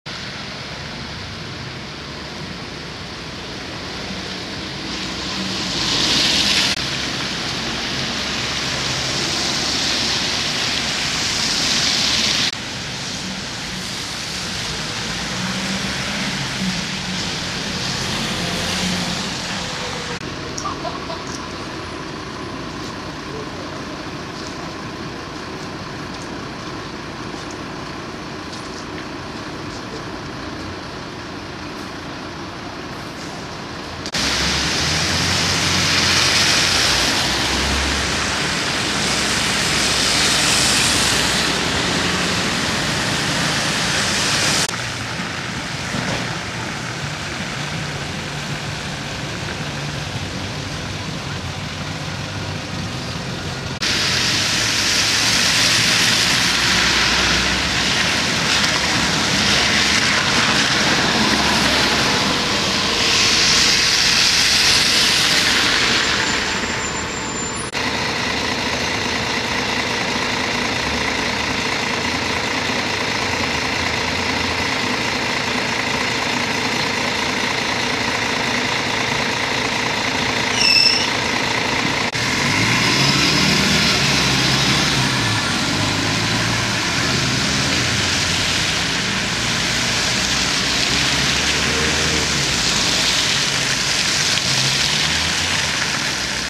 hamburg barmbek fuhlsbüttler straße hamburger im schnee 02.12.2010
ein bisschen schnee,und alles schleicht.mit zum teil sommerbreifung.die fußwege sind natürlich nicht geräumt. alles ist wie immer.